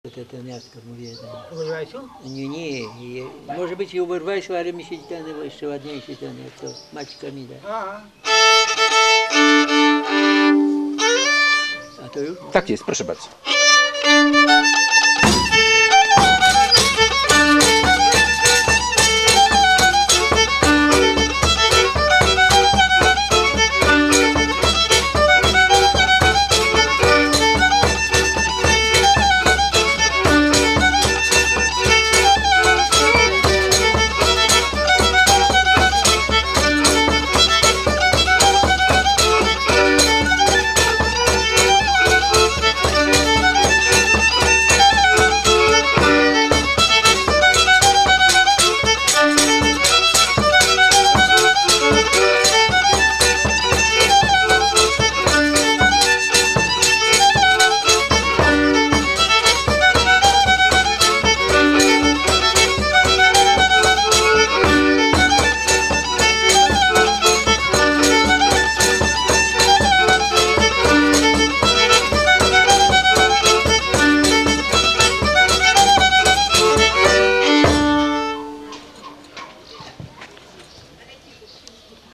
Bronisław Bida skrzypce, Mateusz Cieliszak skrzypce, Czesław Kotuła bębenek. Lubelskie 2002
fundacja Muzyka Odnaleziona/Archiwum Muzyki Wiejskiej
nagrania terenowe Andrzeja Bieńkowskiego
Projekt digitalizacji filmów w 2013 roku dofinansowano ze środków Ministra Kultury
i Dziedzictwa Narodowego oraz przy wsparciu wydawnictwa Muzyka Odnaleziona